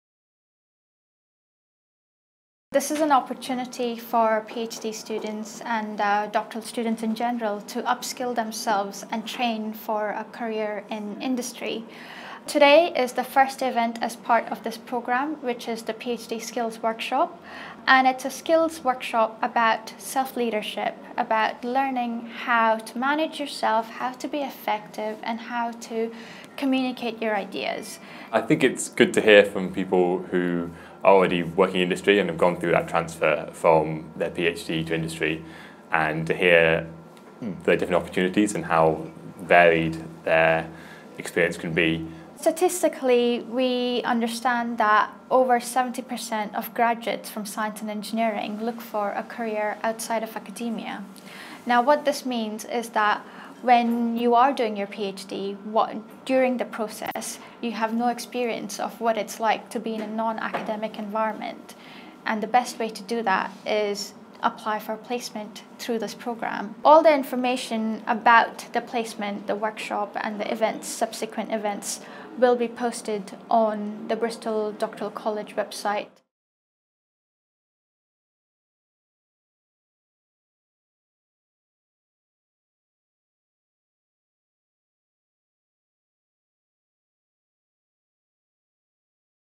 What is the BDC's ‘Skills for Industry’ Programme?
Dr. Aby Sankaran introduces our pilot Industrial PhD Professional Development Programme — a chance for EPSRC-funded doctoral researchers to get relevant real-world experience.